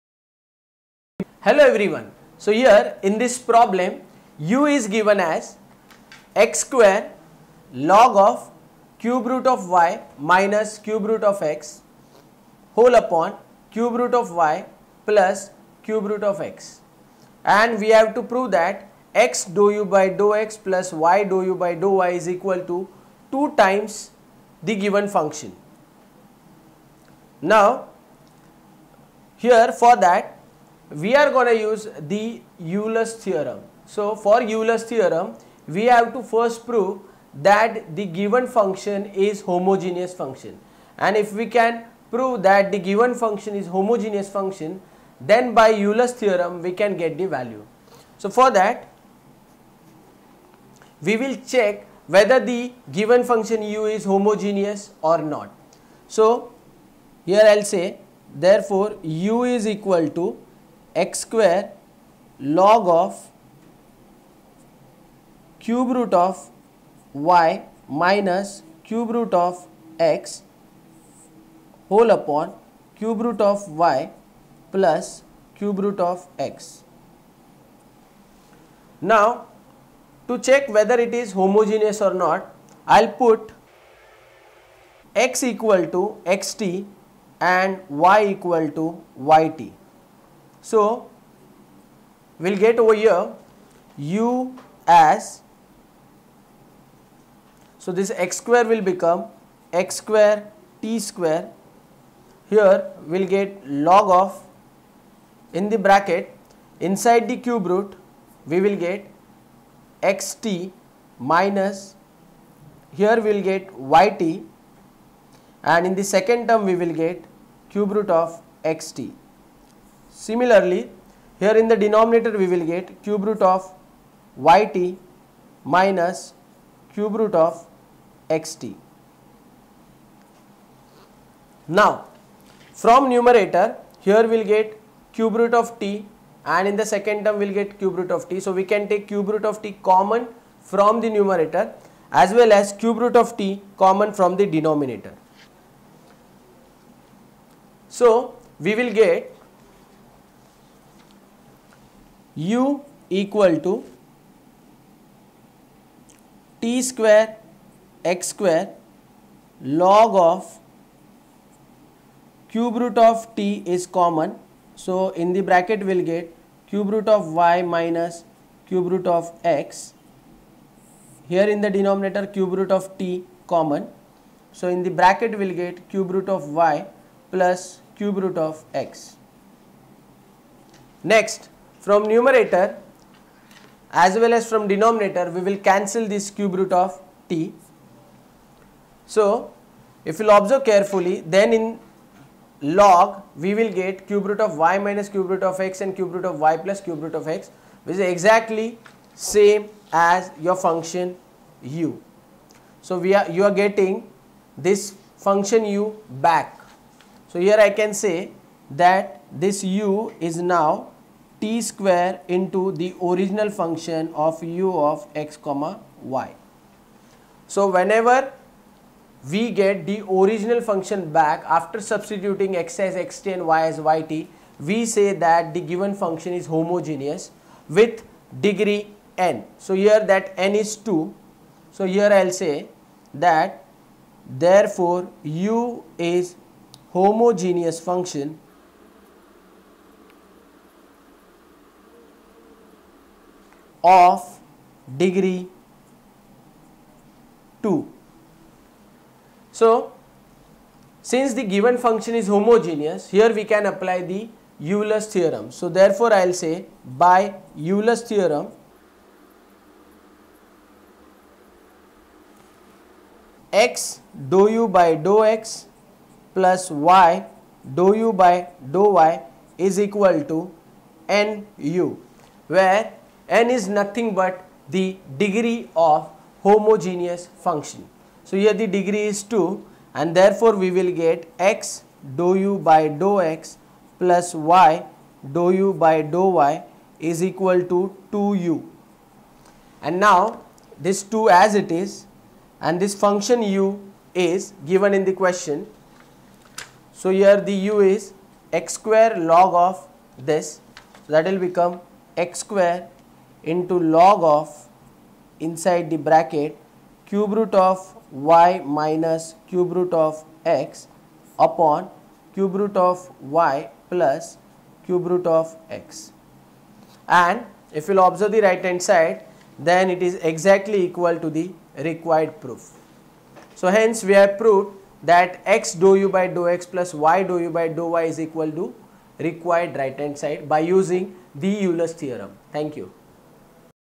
Euler's Theorem Problem No.1 - Homogeneous Functions - Engineering Mathematics 1
Video Name - Euler's Theorem Problem No.1

Chapter - Homogeneous Functions

Watch the video lecture on the Topic Euler's Theorem Problem No.1 of Subject Engineering Mathematics 1 by Professor Mahesh Wagh. Watch the Complete lecture on Engineering Academics with Ekeeda.

Happy Learning.